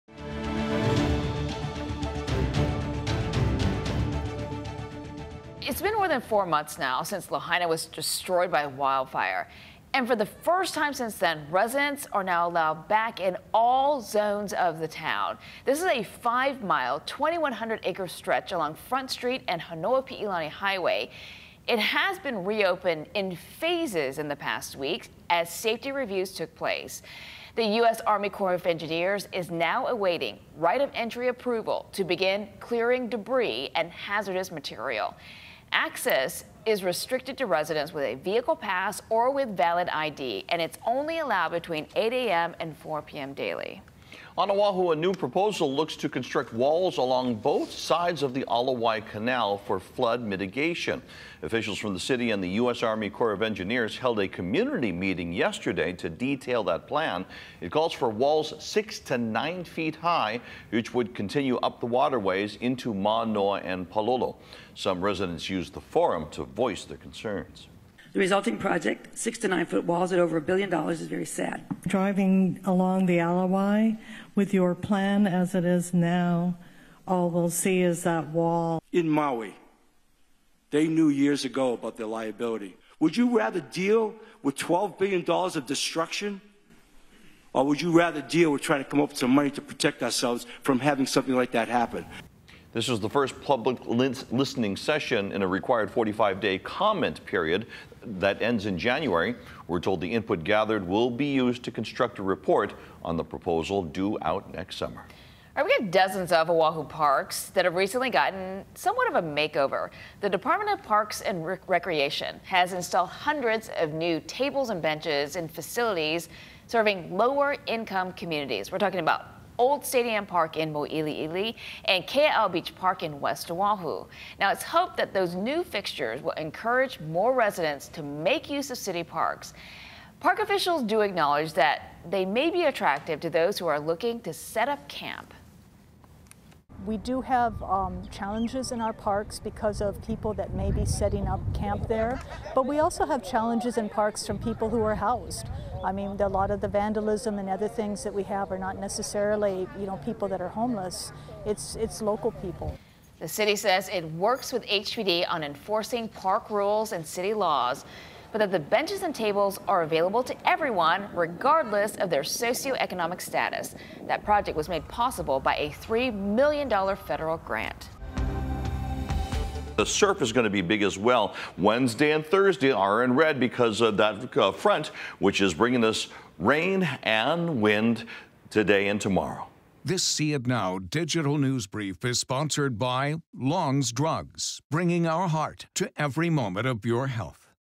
HNN News Brief (Dec. 13, 2023)
Walls along the Ala Wai may be the last thing anybody wants, but engineers say it’s what the city needs to prevent a destructive flood.